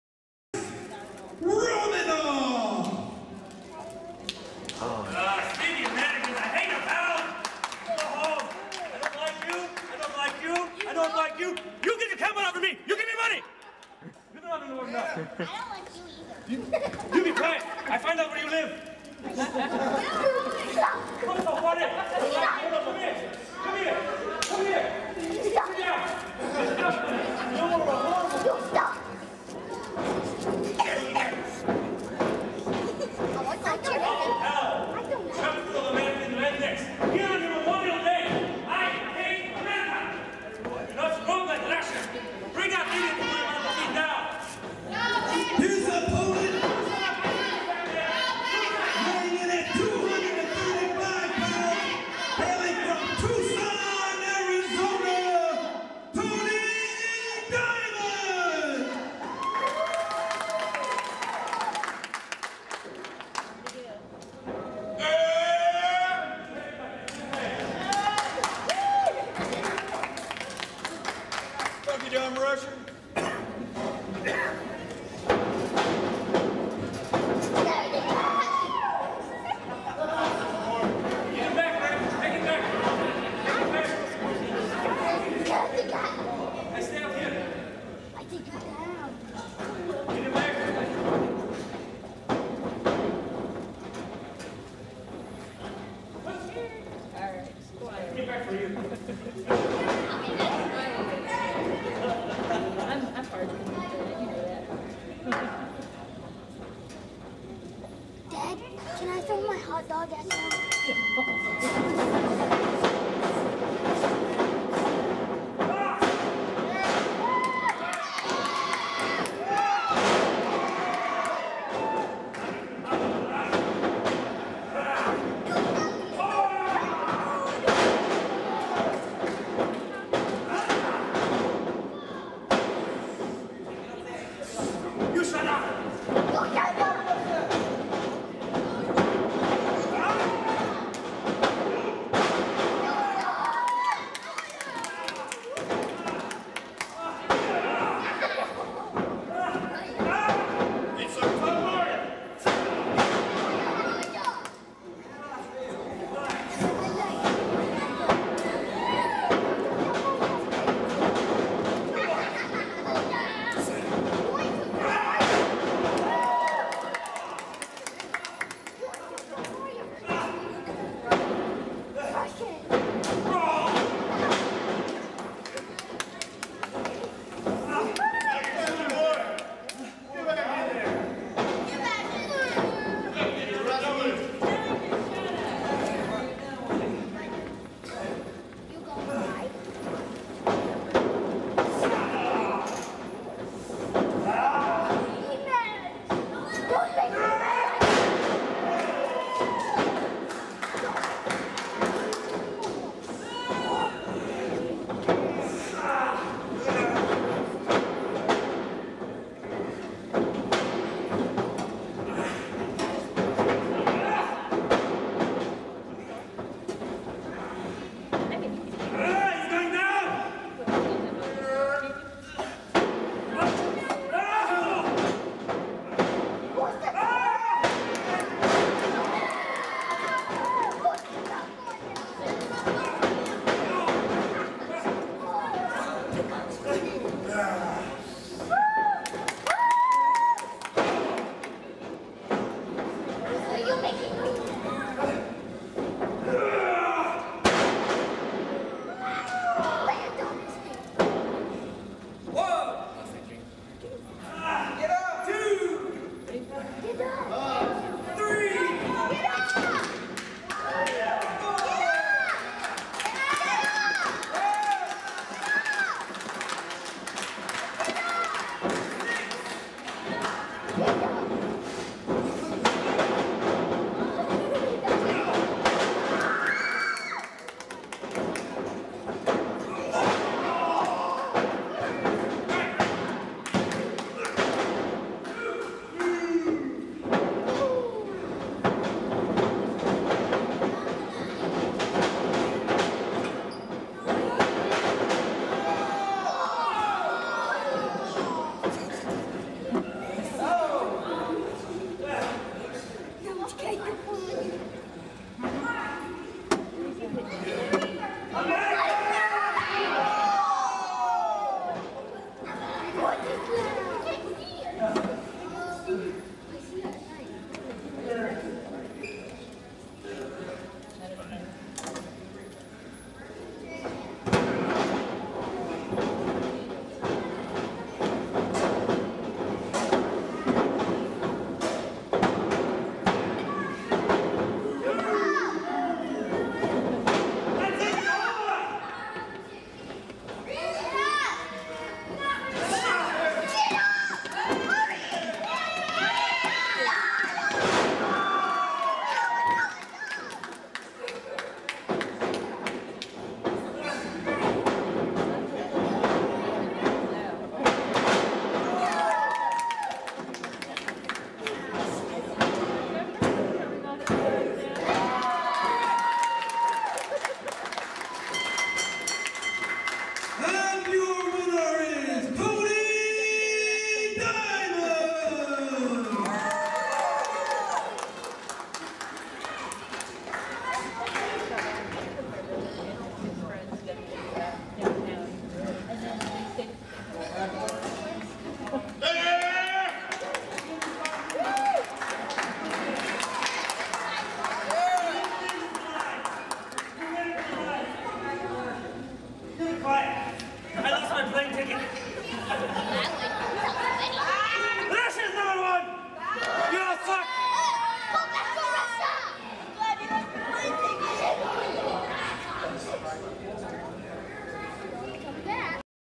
Rex Romanov VS Tony Diamond
Rex Romanov and Tony Diamond face off on Memorial Day 2008.